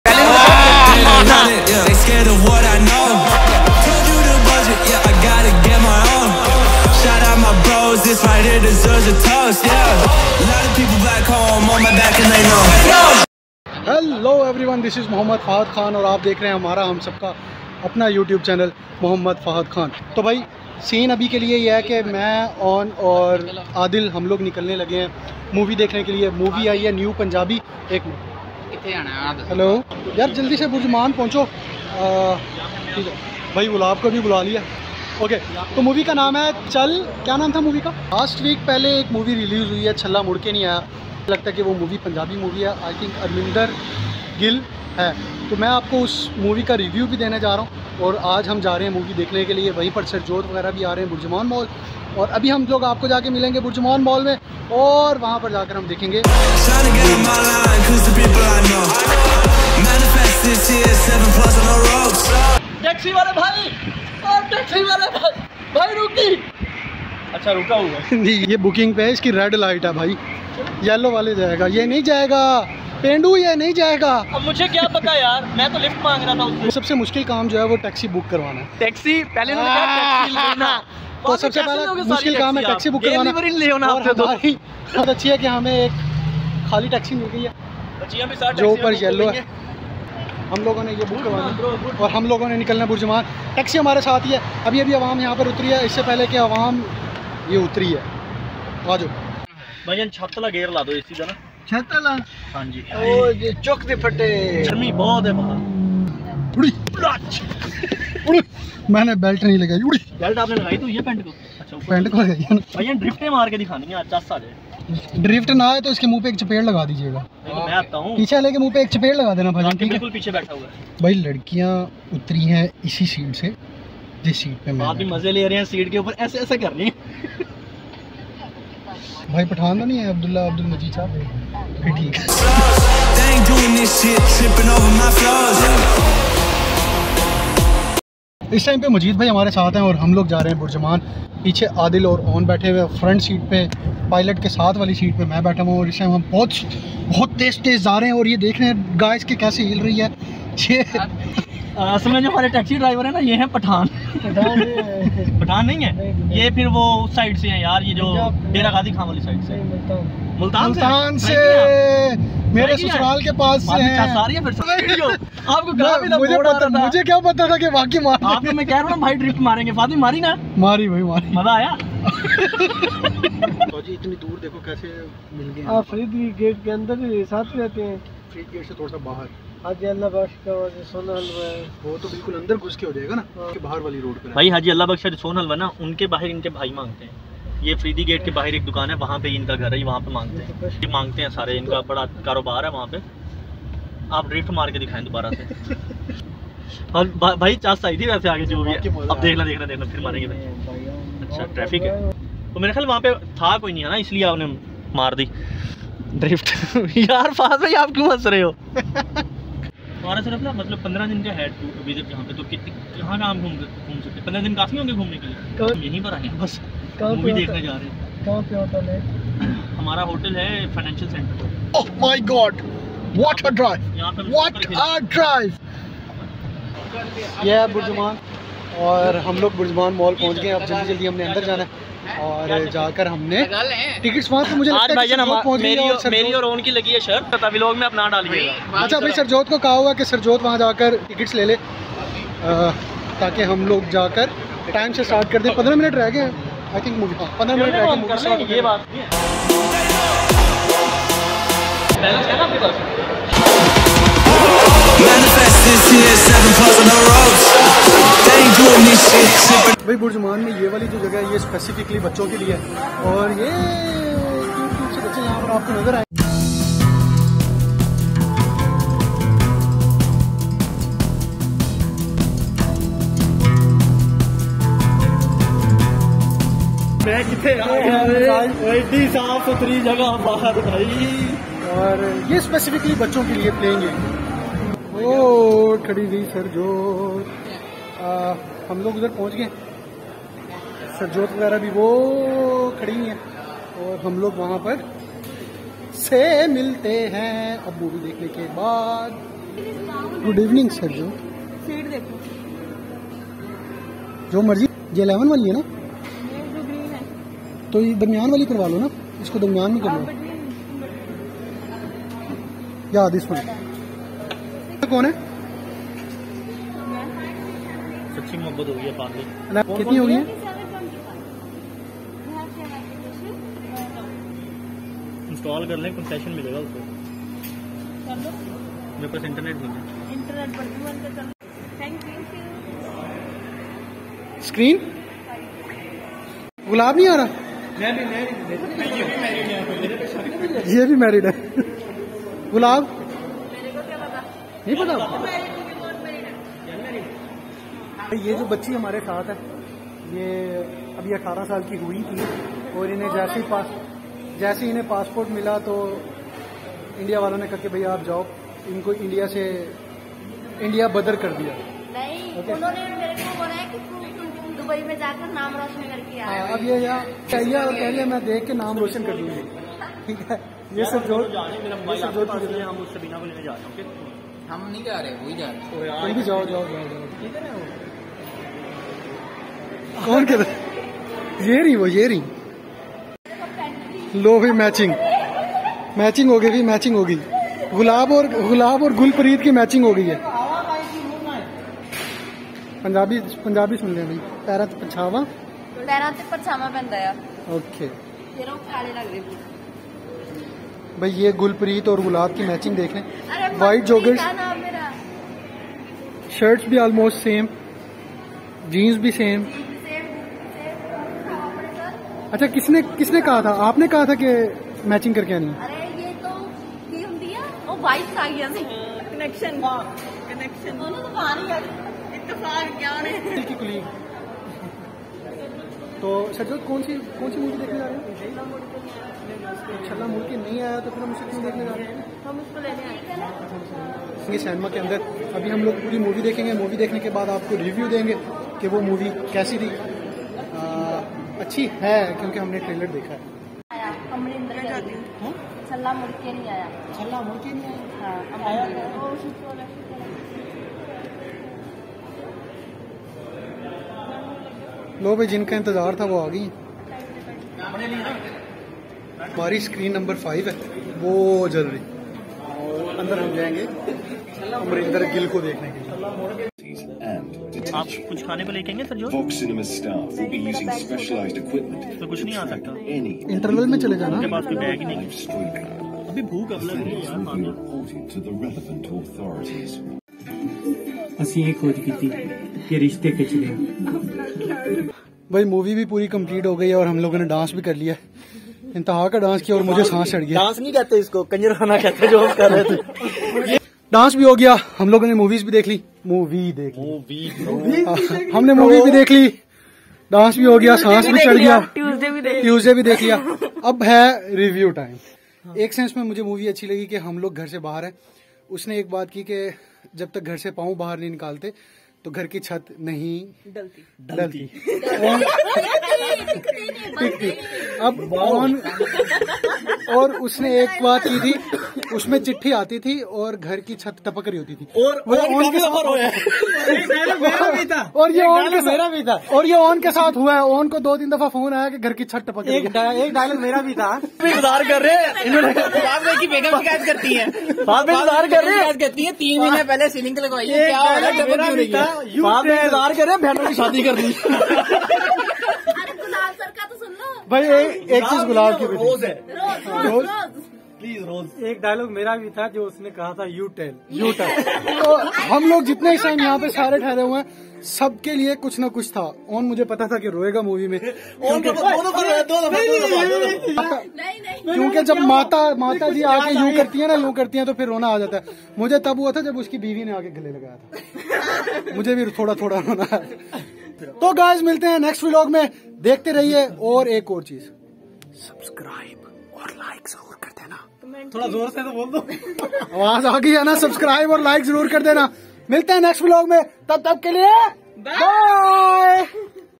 Challa Mud ke Nahi Aaya | Movie Review || Amrinder Gill || Fahad khan vlog
Challa Mud ke Nahi Aaya Movie Review || Amrinder Gill || Punjabi Movie Sargun Mehta | Karamjit Anmol
we watched this movie in dubai cinema, cinema is not too big but over all it is good.
Movie Review Time:
a fantastic movie with good story, but I feel one draw back that was lipsing, but overall sotry is good. amrinder gill who belongs to a poor family visits europ to earn. I think writer tried to add more and more short stories but he failed to windup one.